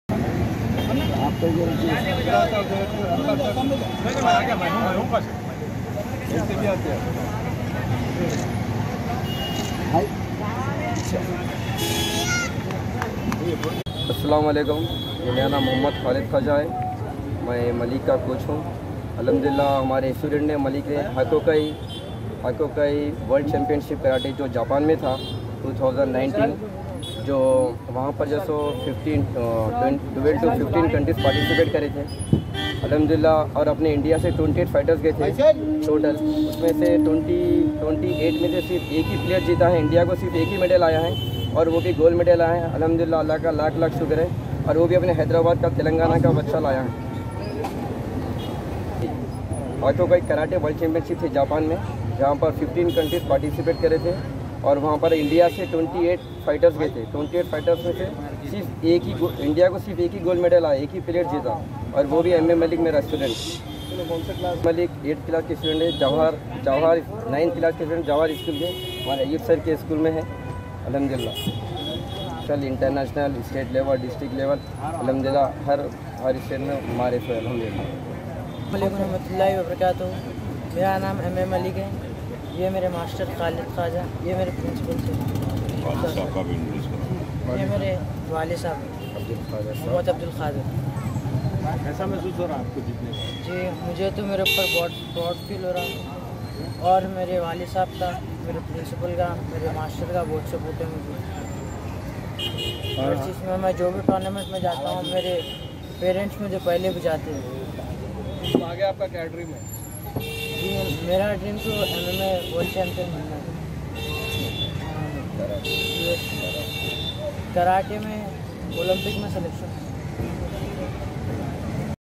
M.A.MALIK WON THE GOLD MEDAL IN KARATE WORLD CHAMPIONSHIP (JAPAN)...NEWS14
NEWS14..M.A.MALIK WON THE GOLD MEDAL IN KARATE WORLD CHAMPIONSHIP (JAPAN)...

(NEWS14 ..NAYYER)